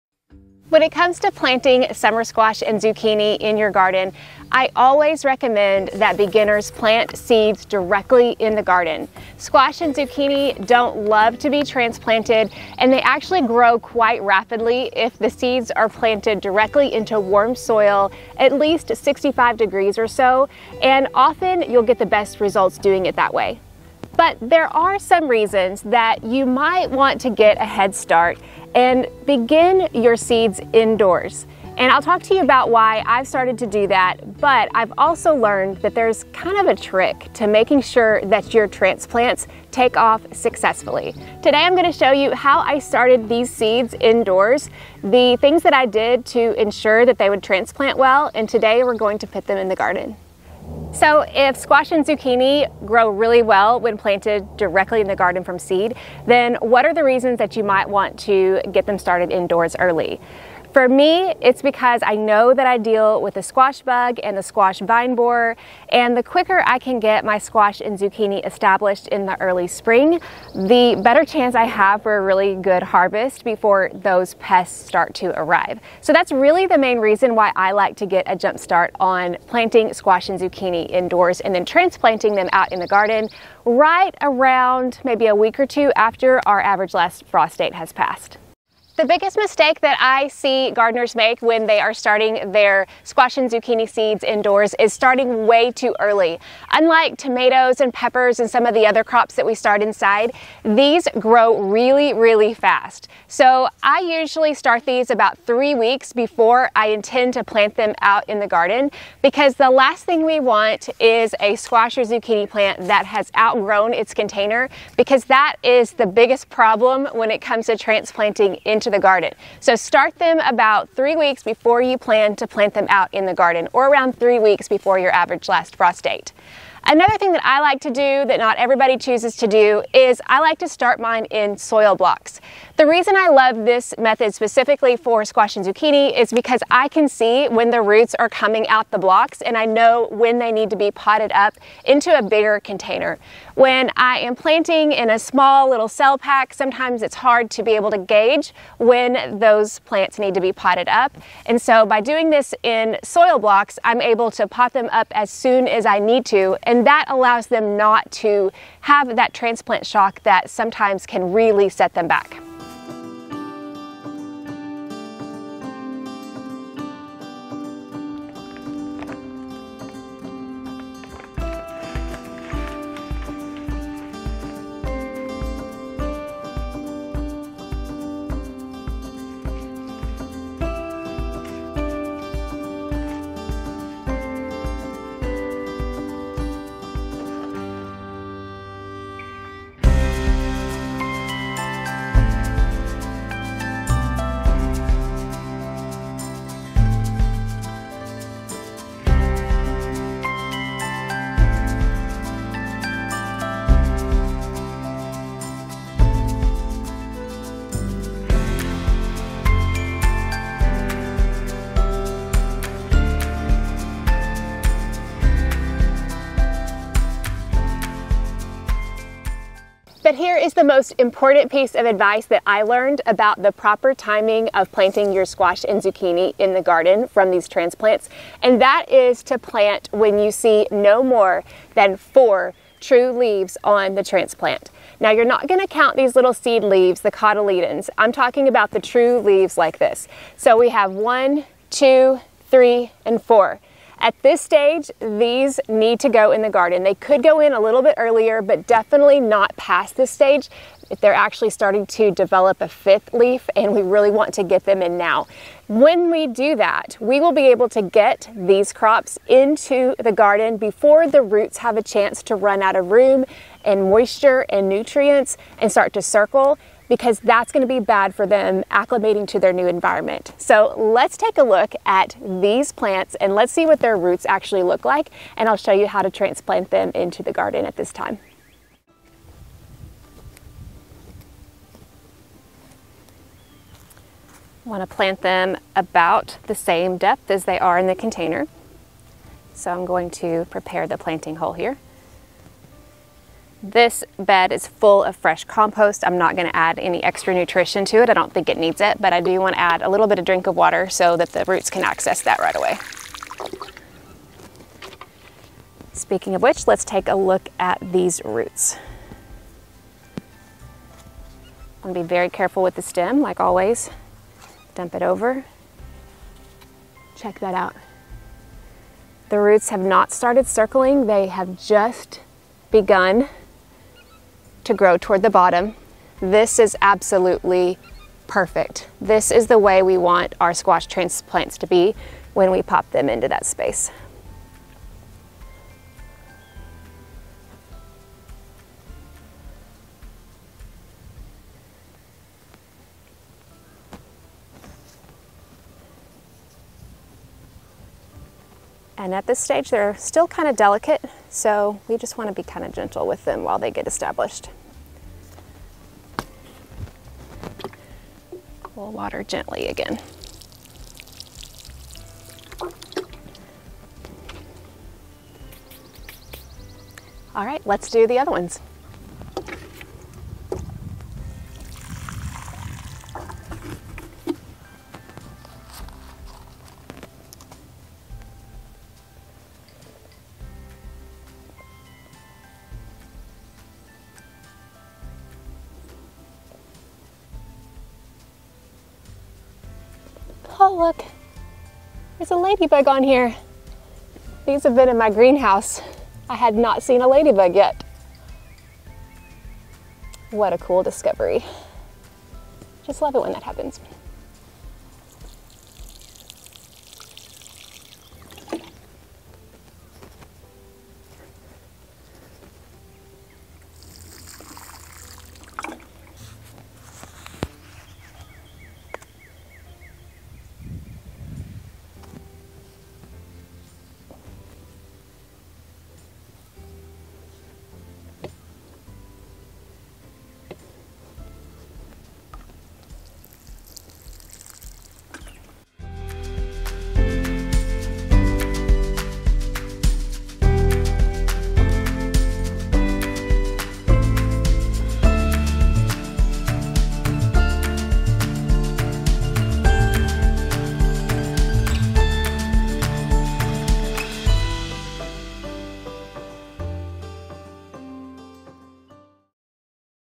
How to Start Squash Indoors (Successfully)
Organic Rev

Soil Blocks - 2:08
Potting up - 2:46
Transplanting - 3:40
Progress - 7:16